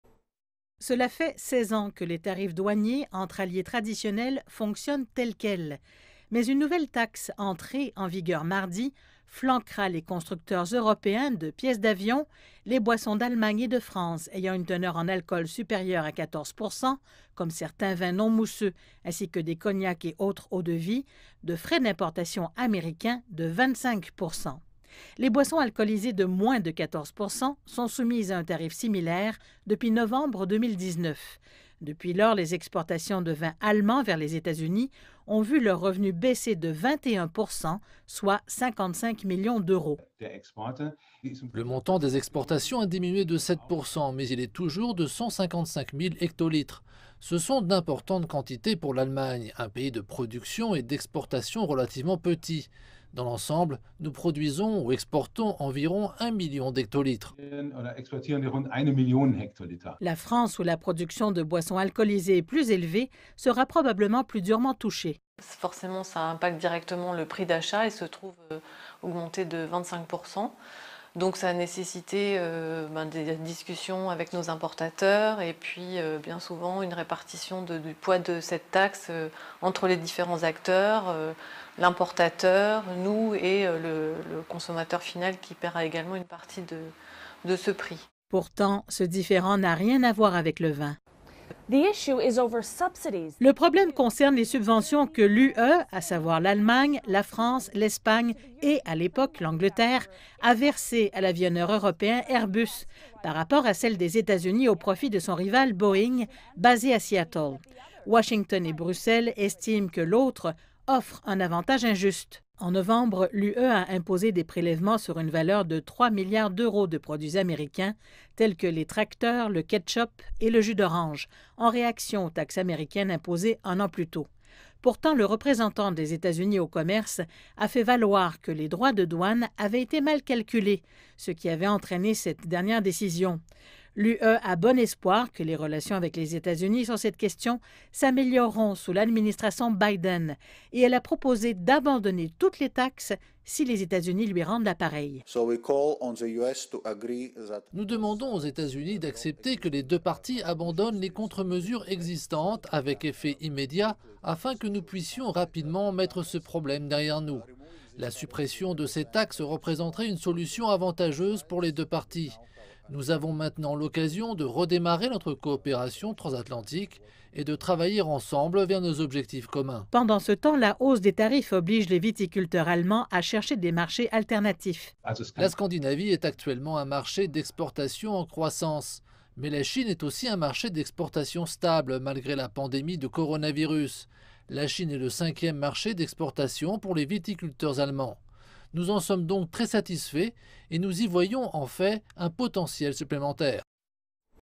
Washington impose une taxe de 25% sur des boissons alcoolisées de l'UE
Cela fait 16 ans que les tarifs douaniers entre alliés traditionnels fonctionnent tels quels. Mais une nouvelle taxe entrée en vigueur mardi, flanquera les constructeurs européens de pièces d'avion, les boissons d'Allemagne et de France ayant une teneur en alcool supérieure à 14%, comme certains vins non mousseux ainsi que des cognacs et autres eaux-de-vie, de frais d'importation américains de 25%. Les boissons alcoolisées de moins de 14% sont soumises à un tarif similaire depuis novembre 2019. Depuis lors, les exportations de vins allemands vers les États-Unis ont vu leurs revenus baisser de 21% soit, 55 millions d'euros.